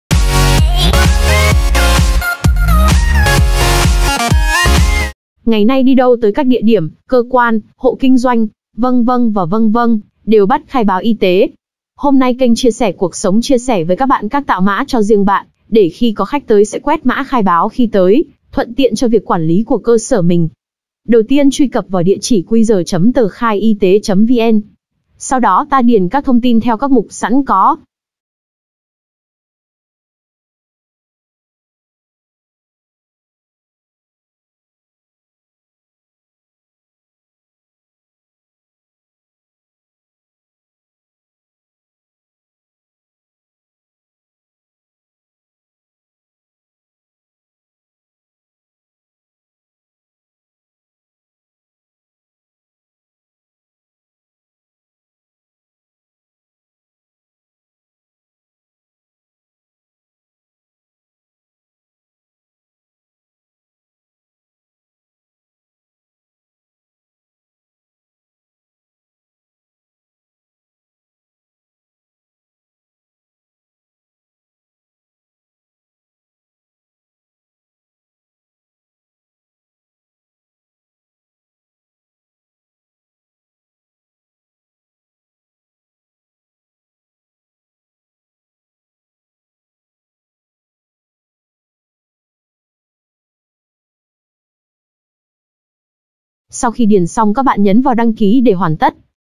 Hướng dẫn tạo mã QR cho doanh nghiệp
- Ngày nay việc khai báo y tế đi đến các địa điểm vị trí nào, đều rất thiết thực và quan trọng, vì khi chúng ta đến đâu sẽ phải quét mã khai báo y tế, để làm cơ sở dữ liệu cho cơ quan chức năng có thẩm quyền theo dõi.
- Hôm nay mình sẽ giới thiệu các bạn cách làm mã QR của c.ty, cửa hàng. của bạn, để mọi người có thể khai báo di chuyển khi đến vị trí đó nhé